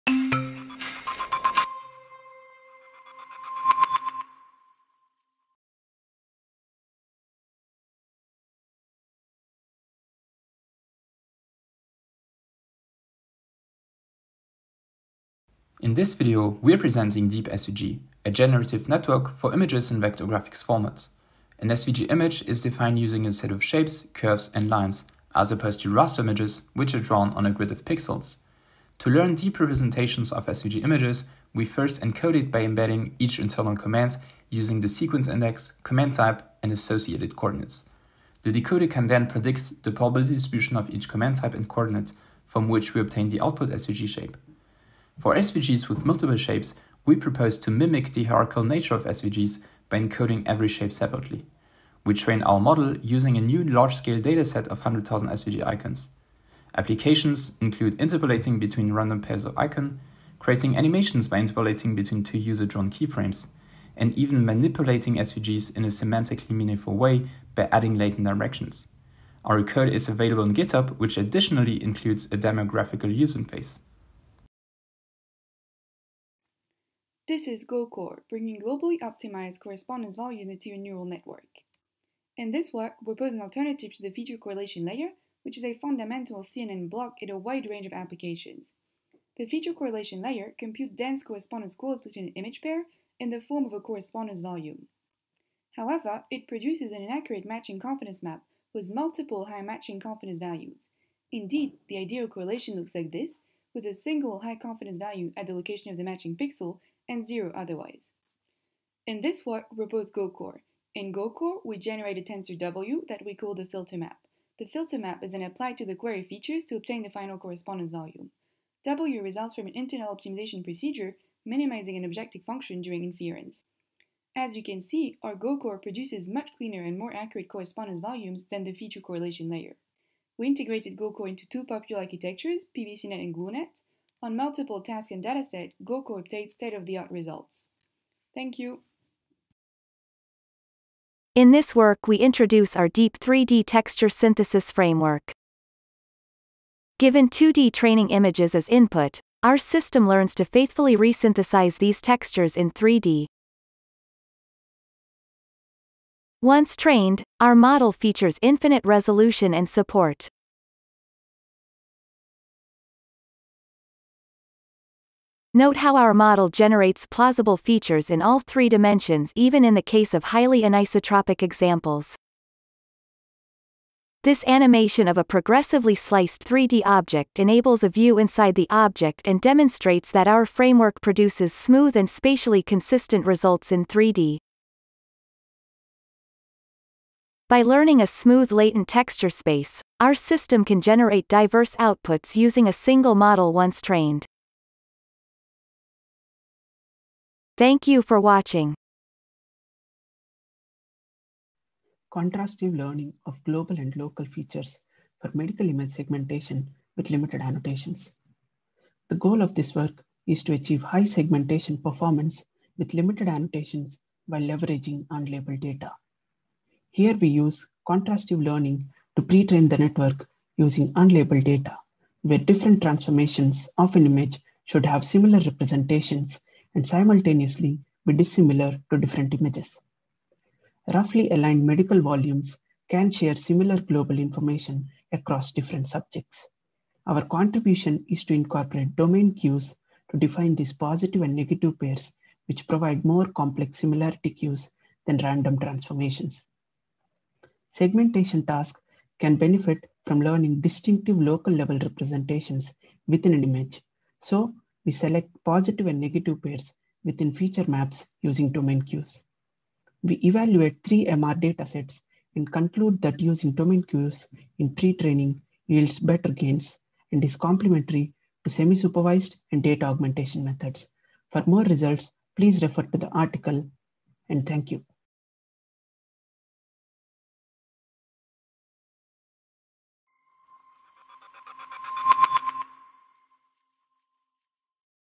NeurIPS Highlights 2020
We are excited to share our NeurIPS2020 papers presented this week from our lab. Check the teaser to get a glimpse of the topics. See you at the poster sessions!